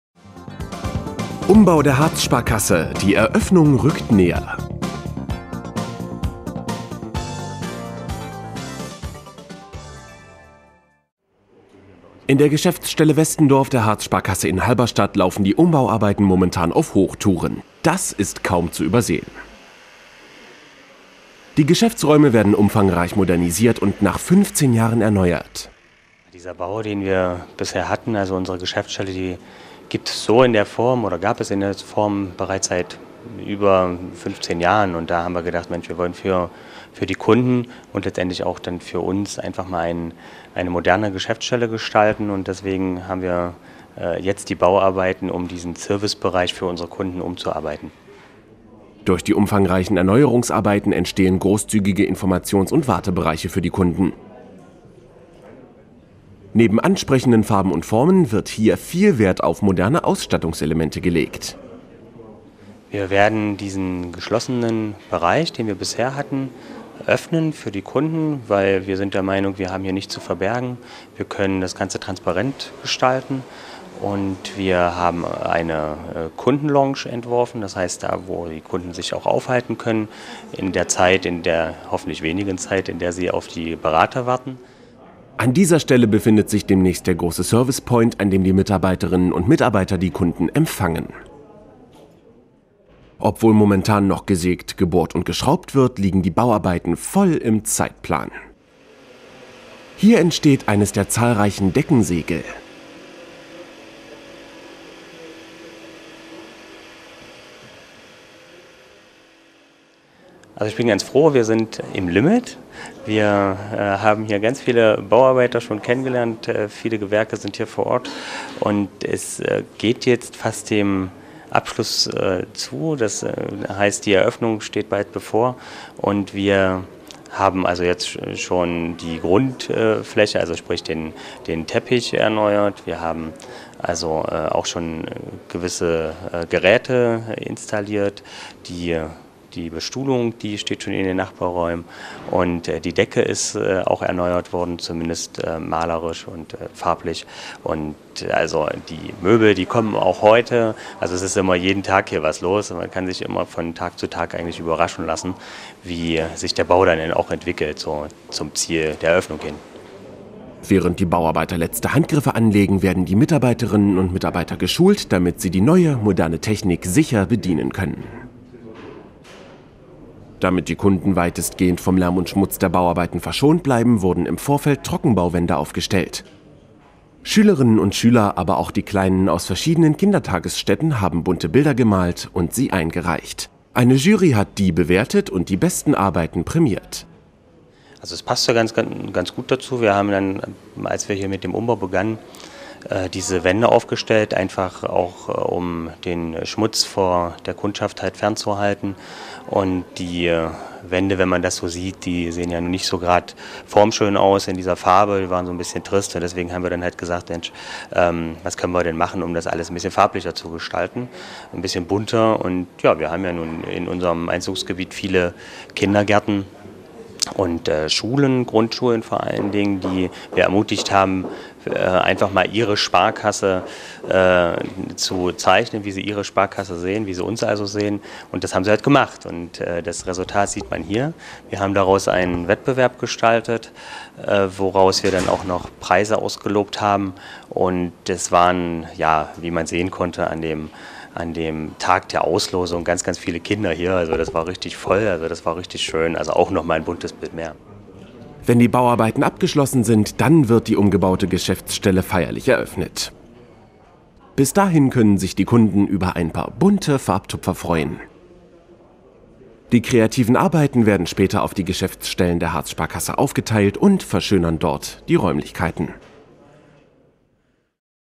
RFH: Umbau Harzsparkasse Westendorf HBS
RFH aktuell: nach 15 Jahren ein neues Outfit für die Geschäftsstelle